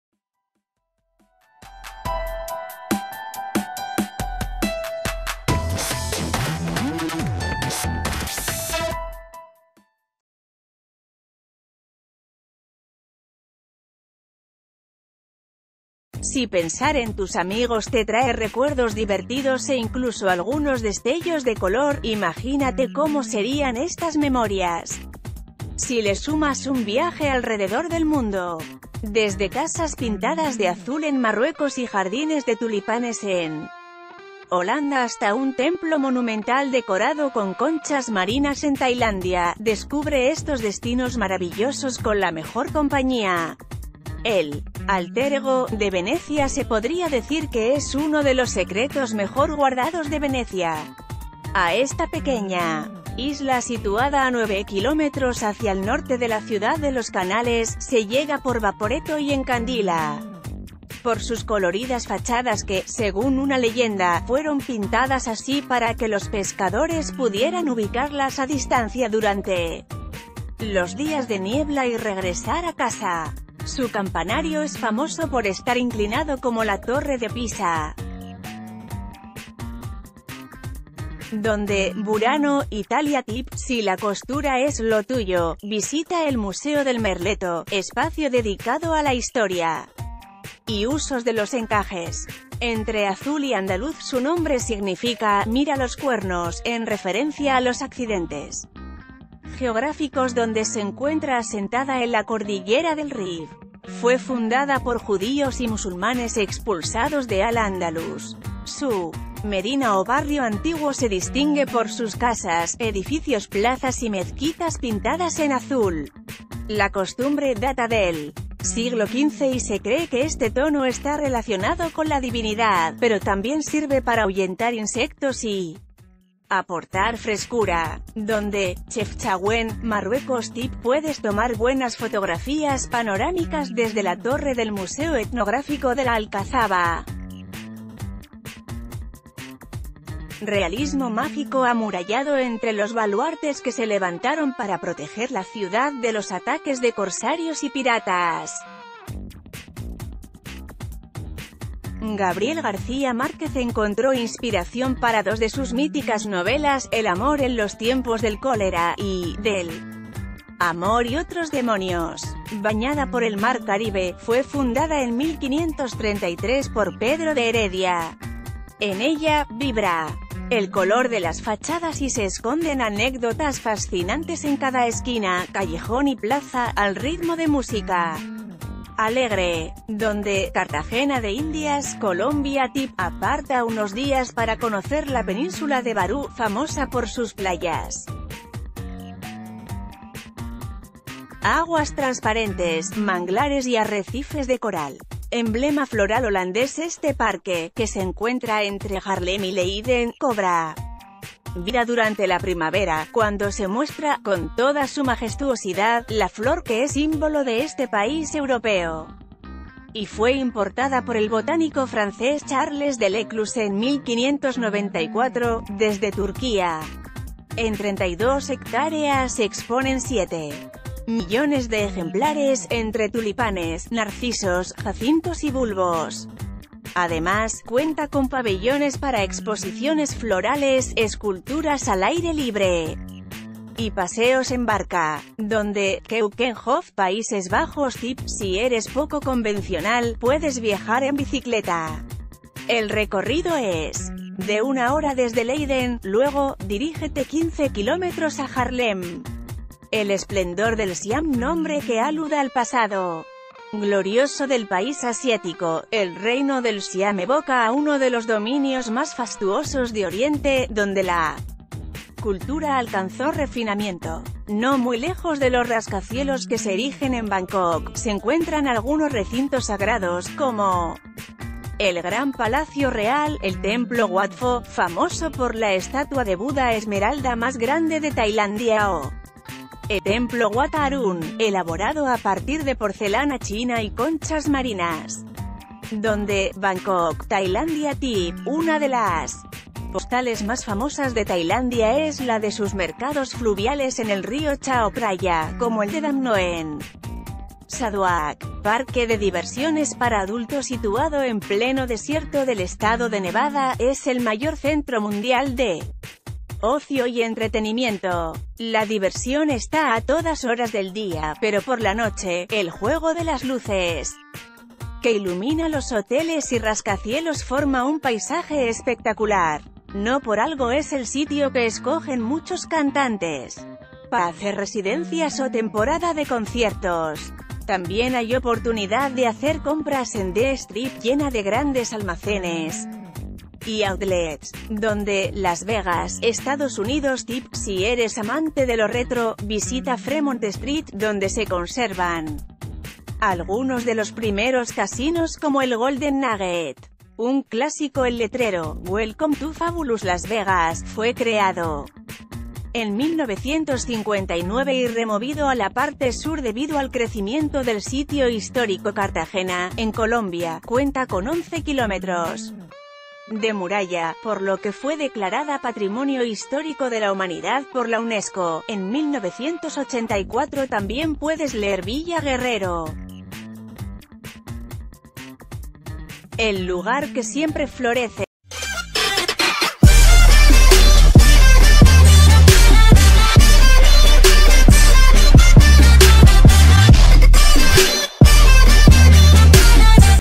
La vuelta al mundo: seis coloridos planes para viajar con amigos
Si pensar en tus amigos te trae recuerdos divertidos e incluso algunos destellos de color, imagínate cómo serían estas memorias si le sumas un viaje alrededor del mundo. Desde casas pintadas de azul en Marruecos y jardines de tulipanes en Holanda hasta un templo monumental decorado con conchas marin...